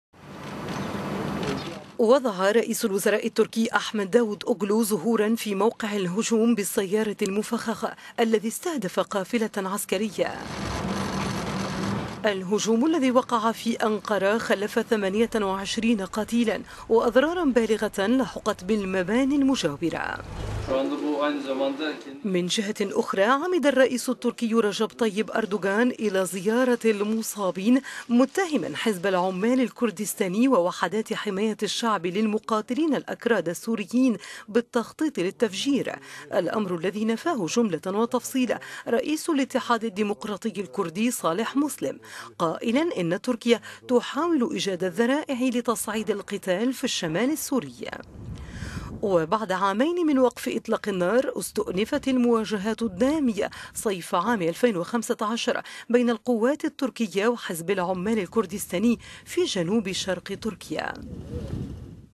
رئيس الاتحاد الديموقراطي الكردي صالح مسلم ينفي وقوف جماعته وراء هجوم أنقره
وضع رئيس الوزراء التركي أحمد داود اوغلو زهورا في موقع الهجوم بالسيارة المفخخة الذي استهدف قافلة عسكرية بأنقره . الهجوم خلف ثمانية وعشرين قتيلا وأضرارا بالغة لحقت بالمباني المجاورة.

من جهة أخرى عمد الرئيس التركي رجب طيب أردوغان إلى زيارة المصابين في المستشفى متهما حزب العمال الكردستاني ووحدات حماية الشعب للمقاتلين الاكراد السوريين بالتخطيط لل تفجير، الأمر الذي نفاه رئيس «الاتحاد الديموقراطي الكردي» صالح مسلم، قائلا إن تركيا تحاول ايجاد الذرائع لتصعيد القتال في الشمال السوري.